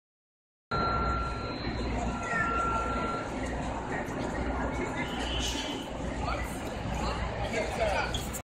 New York City up-close - New York City Public Library, east facade, May 2014
New York City Public Library - main building on Fifth Avenue.